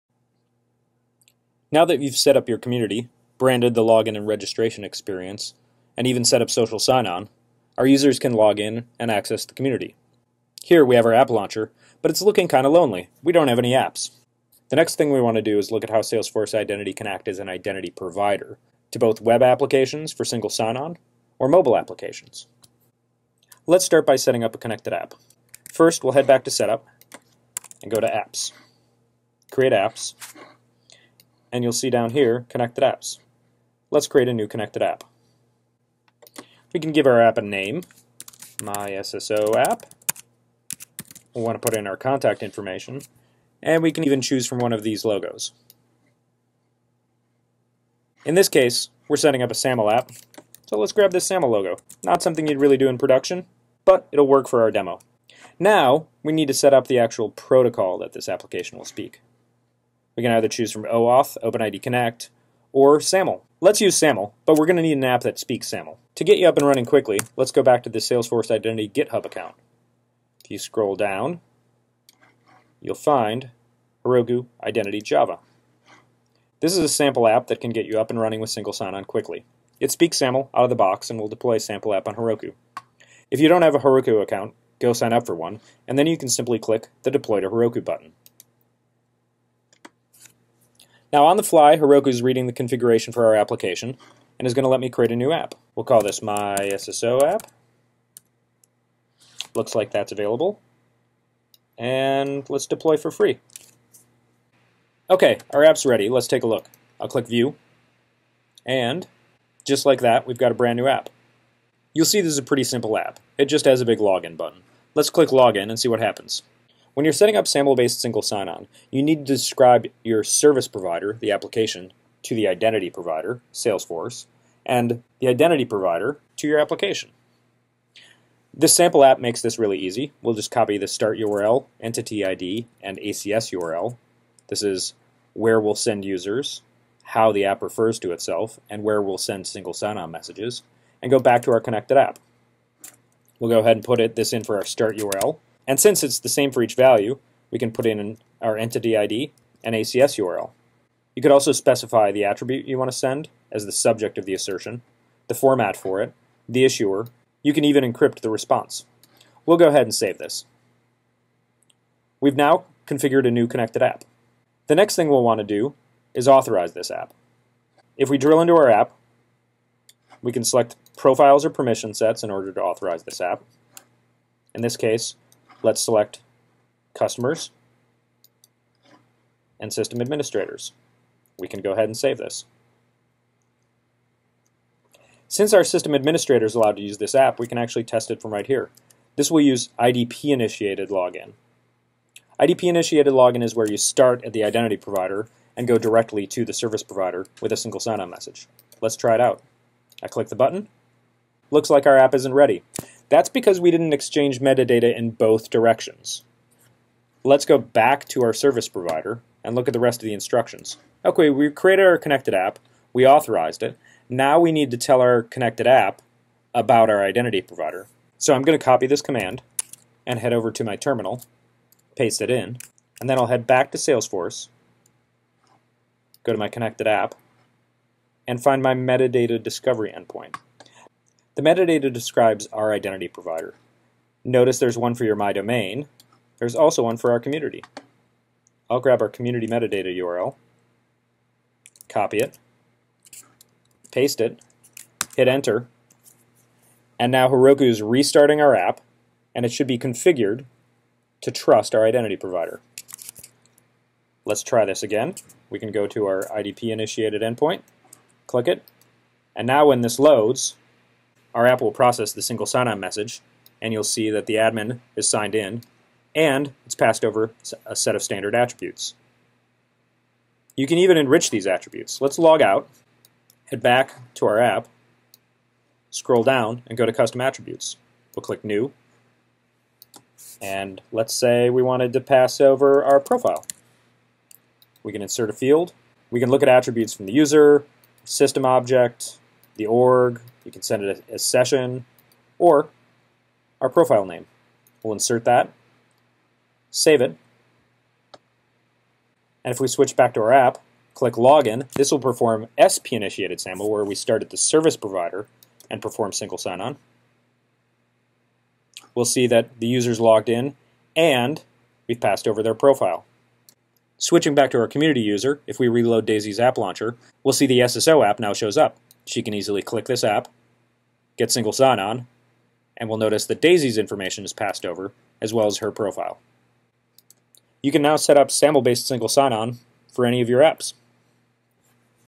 External Identity: How to Setup Single Sign-on With a Sample Application Using SAML | Salesforce
Learn how to make Salesforce Identity act as an Identity Provider in order to provide single sign-on (SSO) to your employees, customers, and partners to other applications using standards like SAML and OpenID Connect. This video will show you how to configure SAML-based SSO using an example app. This video is part of a series about external identity that provides a complete view of how to enable external identity management with Salesforce Identity.